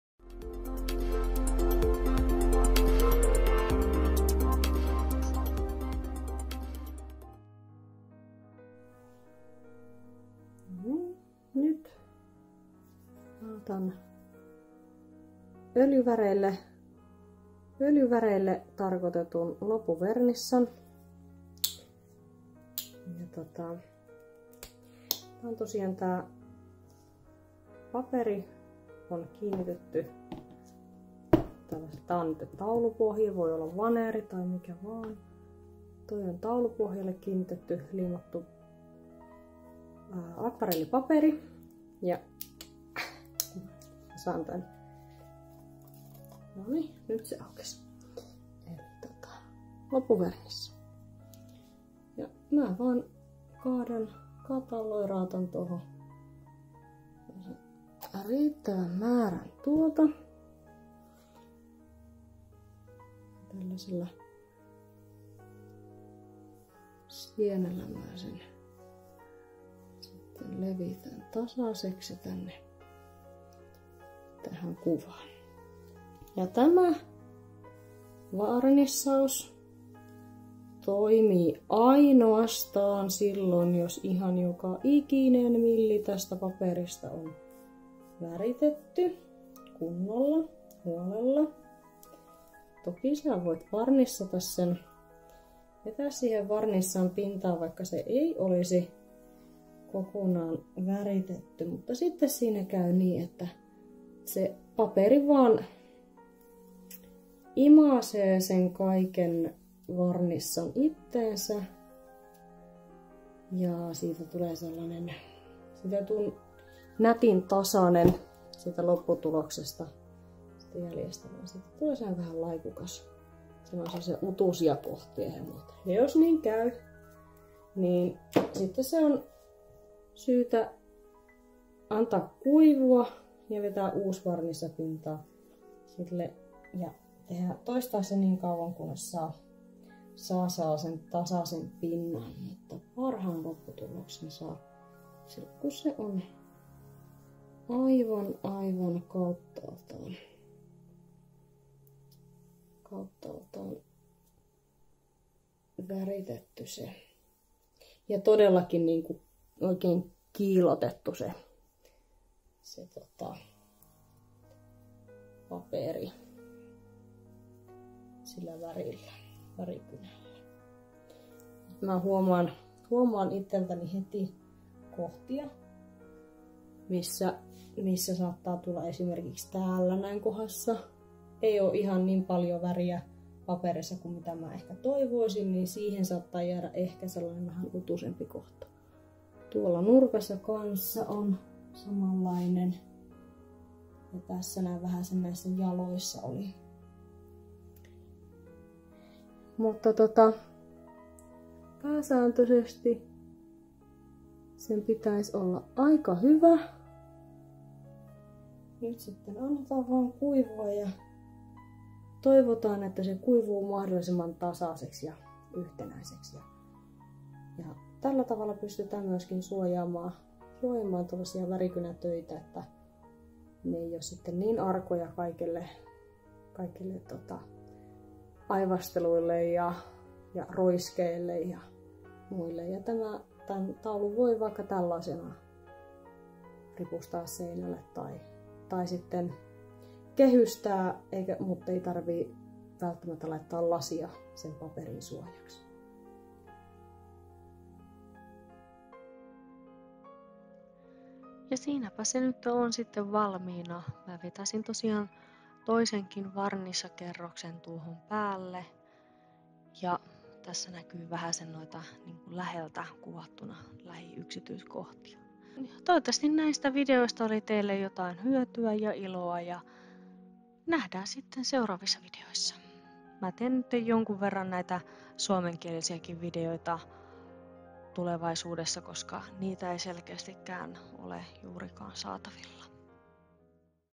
Lentävä peura värikynillä | Osa 5 | Värikynätyö ja varnissan levitys
This video is in Finnish.
Tässä videossa näytän kuinka viimeistelen tämän värikynätyön lakalla, eli Öljyvärimaalauksille tarkoitetulla Varnissalla.

Piirustuksessa käytetyt materiaalit
Paperi: Arches sileä (Hot Pressed) akvarellipaperi 300g
Pohja: Kova taulupohja / Canvas Board
Kynät: Faber-Castell Polychromos, Caran d'Ache Luminance, Derwent Lightfast

@brushandpencilllc3372   Colored pencil Titanium White ja Colored Pencil Touch-up Texture